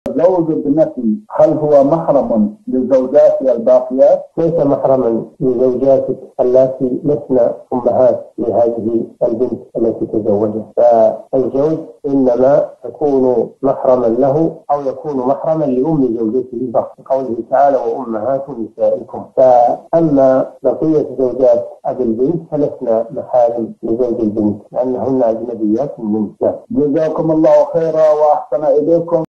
زوج ابنتي هل هو محرم لزوجاتي الباقيات؟ الشيخ صالح بن فوزان الفوزان
نص السؤال:
 زوج ابنتي هل هو محرم لزوجاتي الباقيات؟
السيرة الذاتية :
للاشتراك في قناة الشيخ صالح الفوزان

رئيس الفريق
لمشروع كبار العلماء